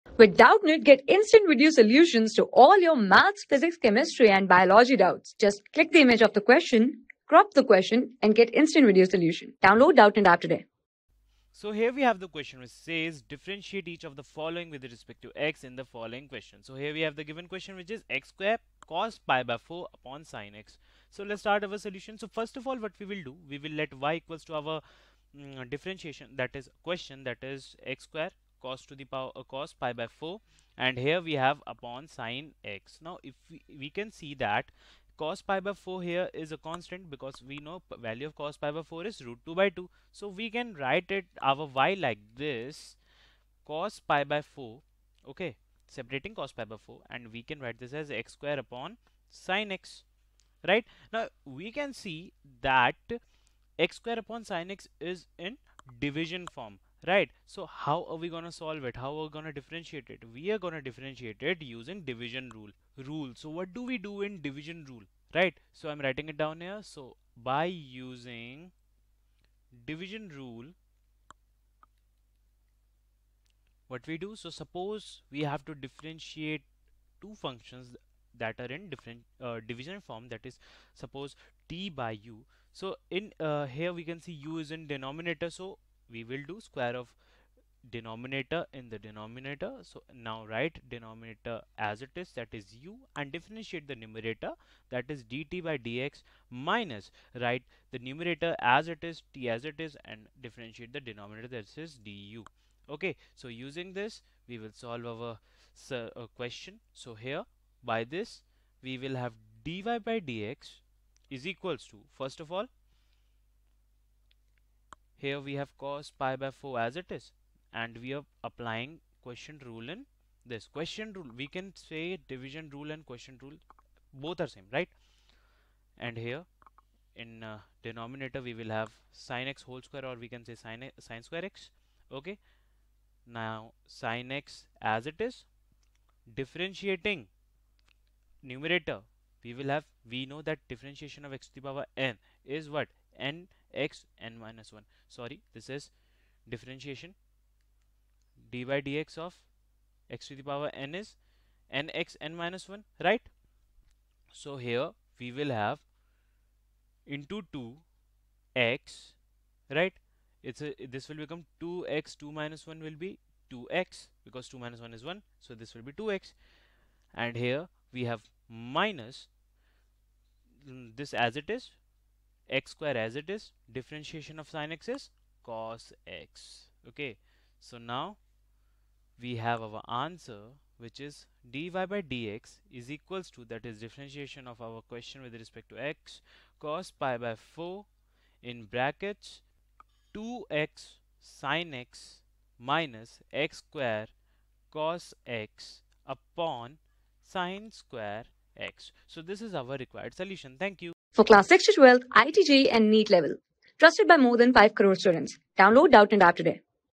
Differentiate each of the following with respect to x in following question: (x^(2)cos(pi)/4)/(s...
Differentiate each of the following with respect to x in following question: (x^(2)cos(pi)/4)/(sinx)
Class: 12
Subject: MATHS
Chapter: DIFFERENTIAL CALCULUS
Board:IIT JEE

👉 Have Any Query? Ask Us.